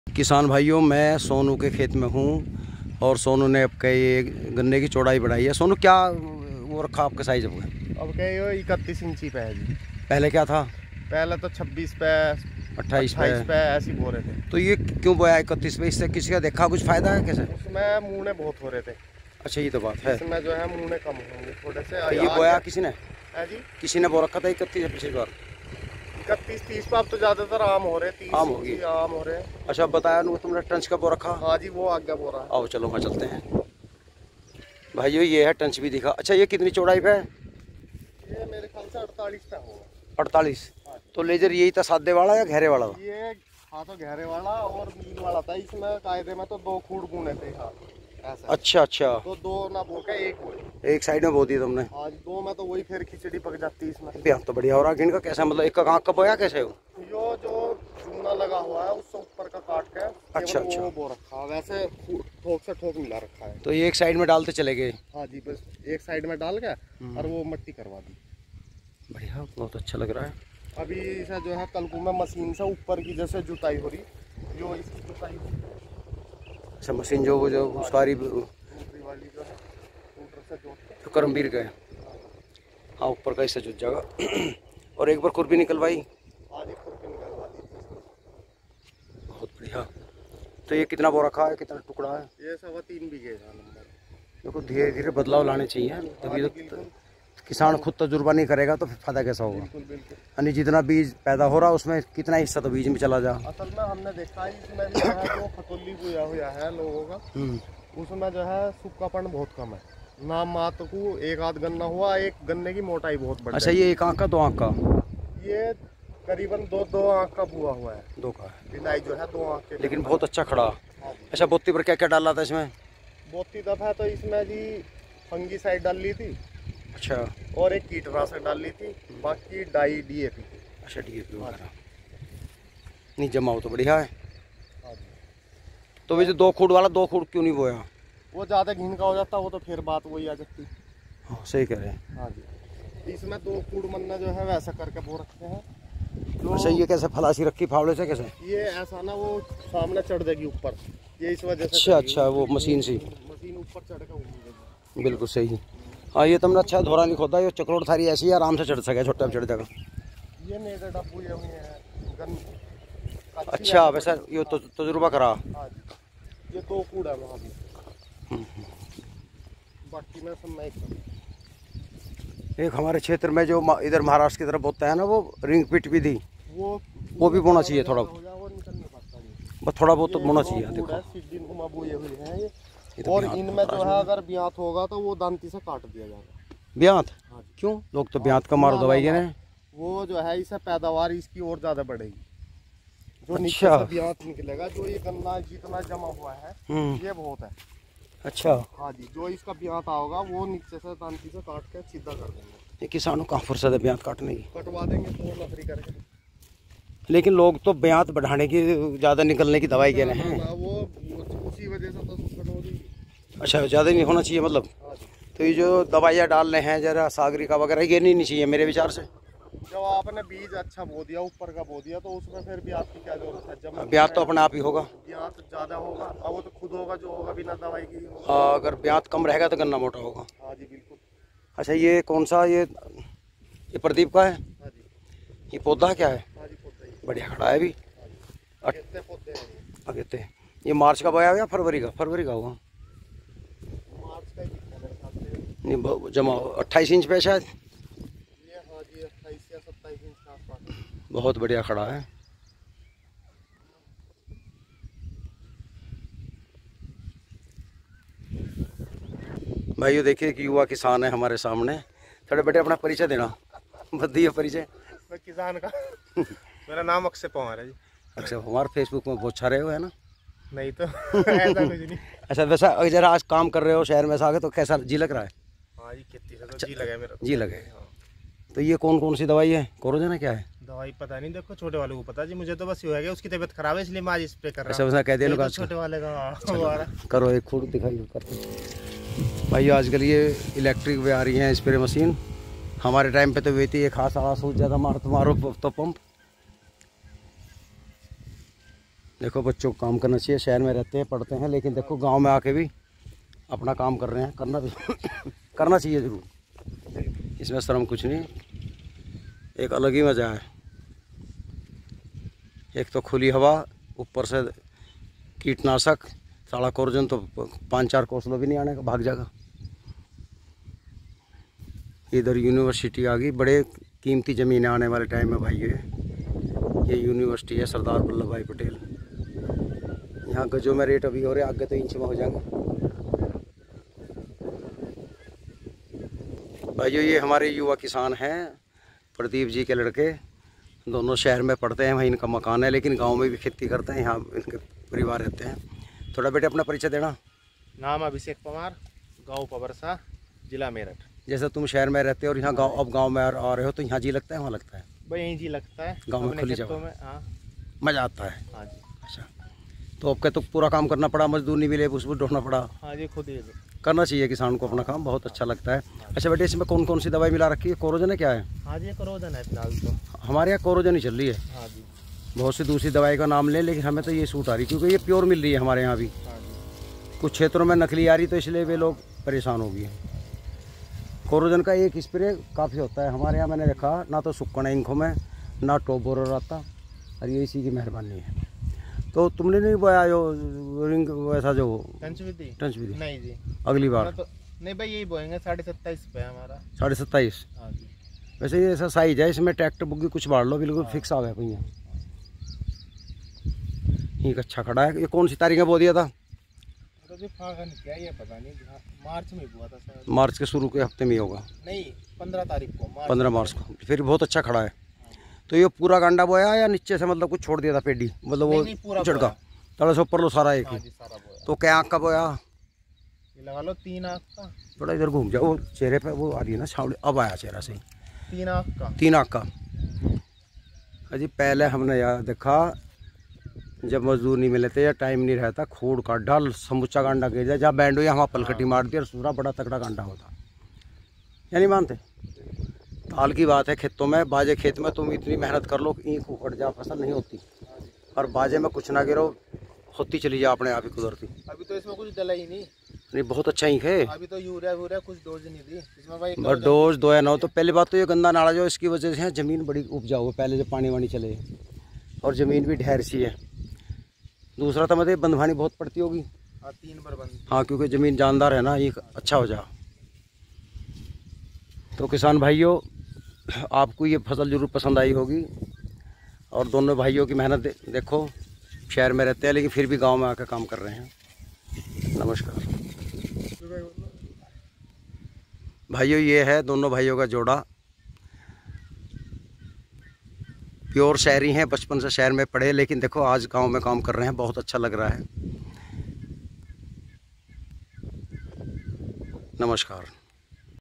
युवा बने किसान //खास जानकारी 29 inch और ट्रांस विधि का गन्ना बोया
किसान भाइयों सोनू का कहना है गन्ने का बयात बढ़ाने की दवाई कभी नहीं डालनी चाहिए सोनू का मानना है गन्ना बारीक रह जाता है